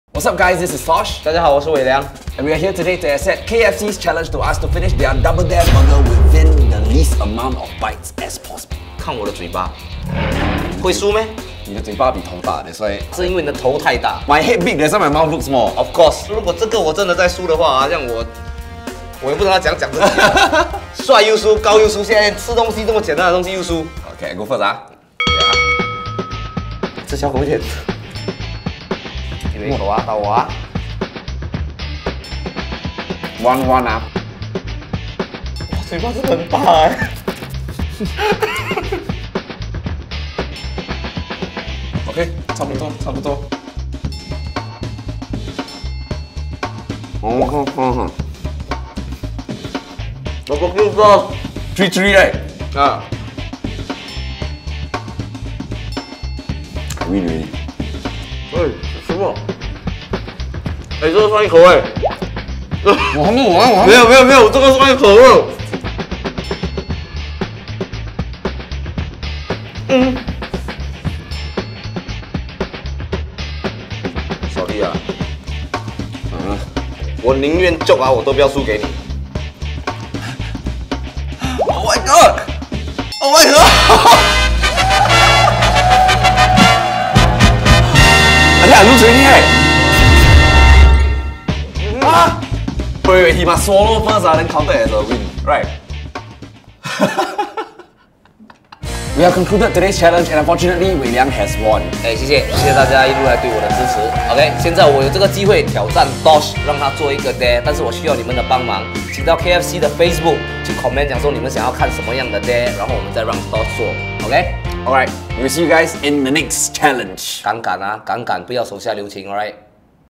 KFC Double Daredevils - Big Mouth Challenge (Tosh & Weiliang)
How many bites do Tosh and Weiliang need to finish the epic KFC Zinger Double Dare? Watch them in action here and see who is the winner of the Big Mouth Challenge!
You can suggest the next dare challenge for Tosh to do